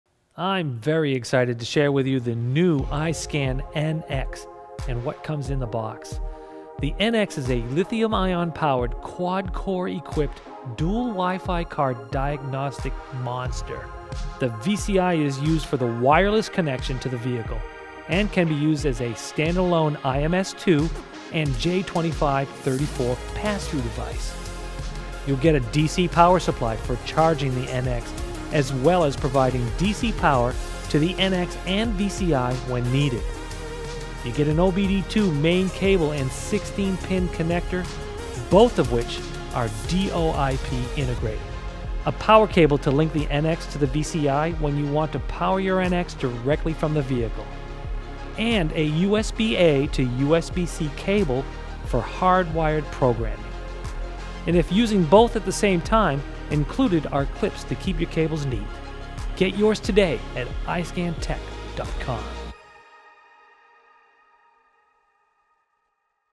iSCAN NX - What Comes in The Case - Unboxing Explained
The new iSCAN NX and what comes in the box.

The NX is a lithium ion powered, quad core equipped, dual Wi-Fi card diagnostic monster / platform

The VCI is used for the wireless connection to the vehicle and can be used as a standalone IMS2 and J2534 pass-thru device

You’ll get a DC power supply for charging the NX as well as providing DC power to the NX and VCI when needed.

You get an OBD II main cable and 16 pin connector, both of which are DoIP integrated.

A power cable to link the NX to the VCI when you want to power your NX directly from the vehicle

And a USB A to USB C cable for hardwired programming

And if using both at the same time, included are clips to keep your cables neat.